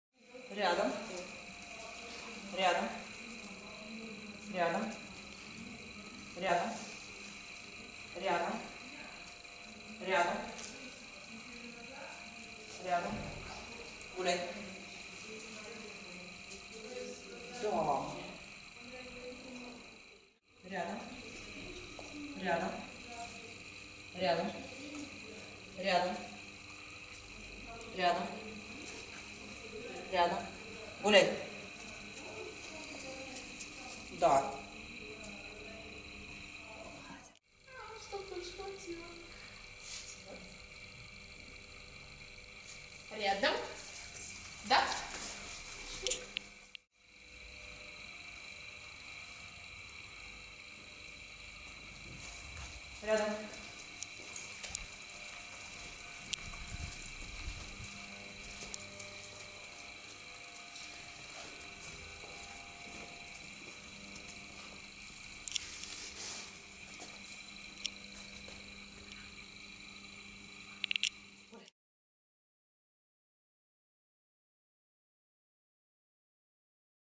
Татьяна и хаски Бакки. Занятие по дрессировке: обучение собаки движению рядом. Тольятти, 5 марта '20
Татьяна и хаски Бакки.
Занятие по дрессировке (группа начального послушания): обучение собаки движению рядом.
Тольятти, 5 марта 2020.
Дрессировка собак в Тольятти
тел. и WA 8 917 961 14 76
тел. и Viber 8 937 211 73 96